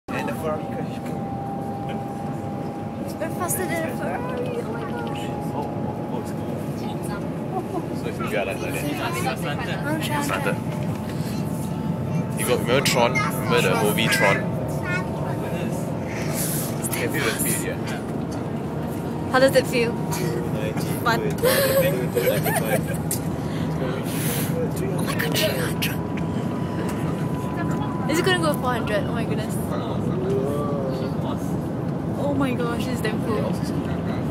Maglev Train in Shanghai
Our first ride on the Maglev Train